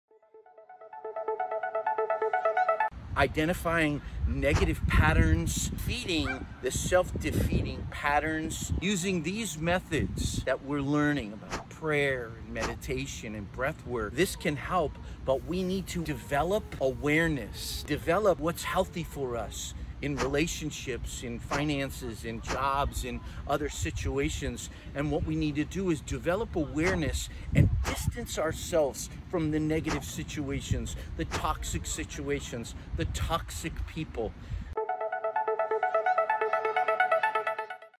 Unlock Positivity: Overcoming Negativity with John Charles Grigg Teaser!
Dive into a world of positivity with John Charles Grigg! 🚀 In this teaser, discover actionable insights on overcoming negativity and embracing a brighter perspective. Join the journey towards a positive mindset that empowers and uplifts. Don't miss out on the transformative wisdom! 💡✌️